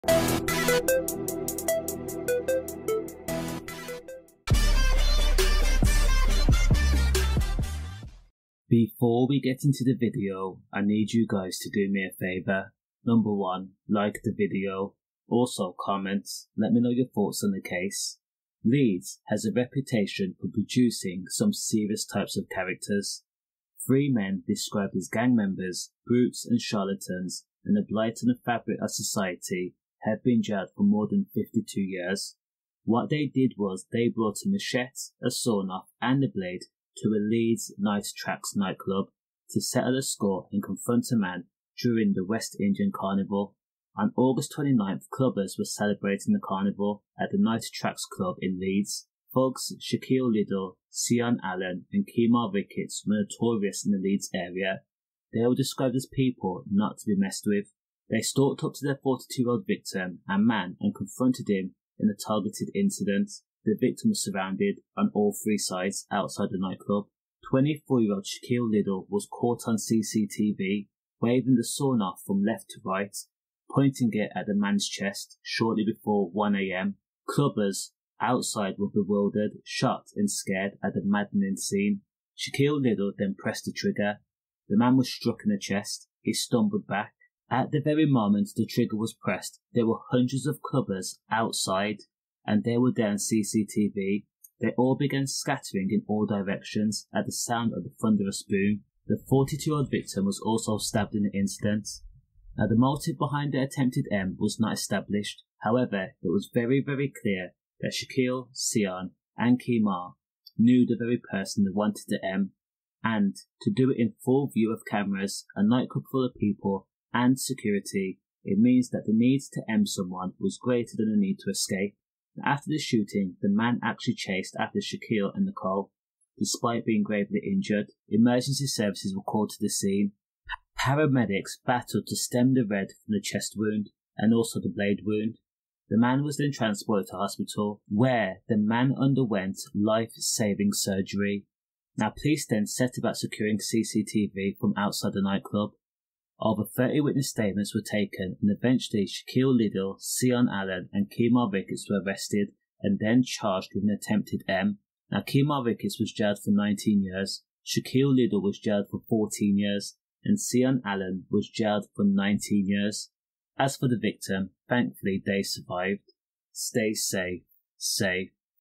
Leeds gang sh**t and st*b man outside nightclub in front of people
leeds gang was jailed for more than 50 years  after CCTV cameras also filmed the 42 year-old year-old victim being bladed  in the chest during the "appalling" incident which happened in sight of revellers celebrating last year's Leeds West Indian Carnival weekend. He was also sh*t with a sawn off at point blank range

Shaquille Liddie, 24 and from Leeds, was identified as the g*nman and was found to have shot the man in the chest when he was just a few feet away from him.

He and his accomplices Kemar Ricketts, 29, and Seion Allen, 33, were tracked down following an extensive investigation by Yorkshire Police detectives.
they were jailed for between 14 and 19 years each.